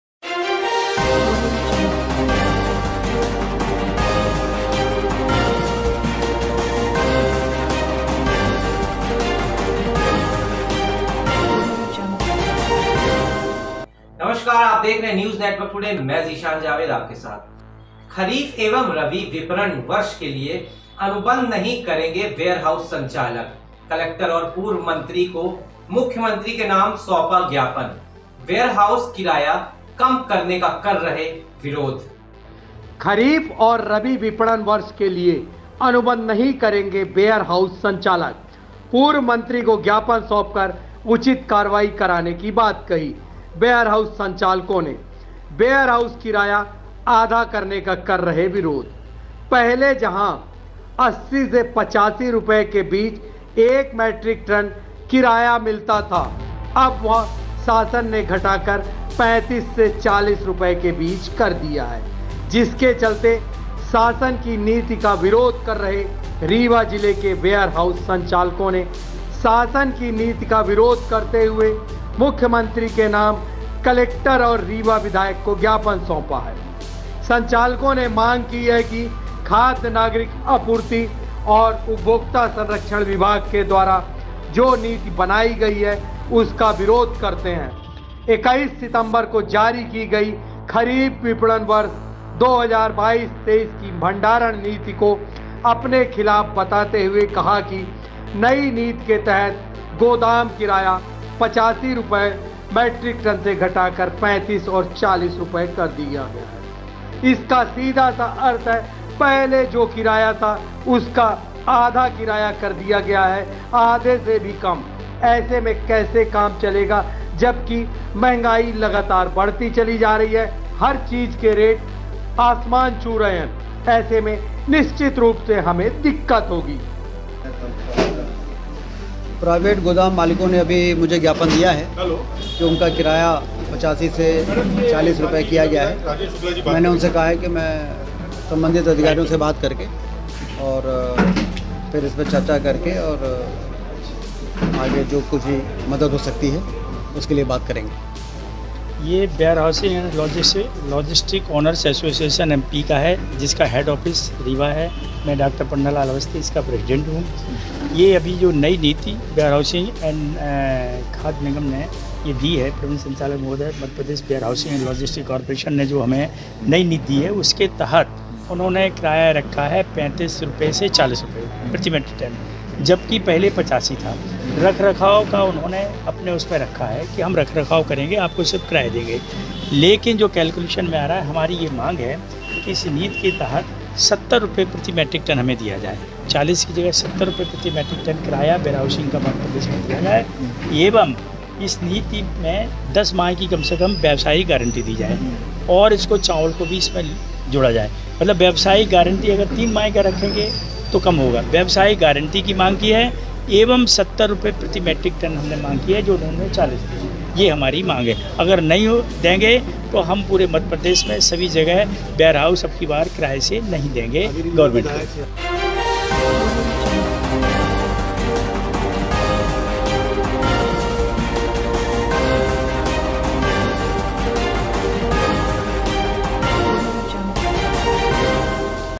खरीफ एवं रबी विपणन वर्ष के लिए अनुबंध नहीं करेंगे वेयरहाउस संचालक पूर्व मंत्री को ज्ञापन सौंपा
खरीफ एवं रबी विपणन वर्ष के लिए अनुबंध नहीं करेंगे वेयरहाउस संचालक पूर्व मंत्री को ज्ञापन सौंपकर उचित कार्यवाही कराने की बात कही वेयरहाउस संचालकों ने वेयरहाउस किराया आधा करने का कर रहे विरोध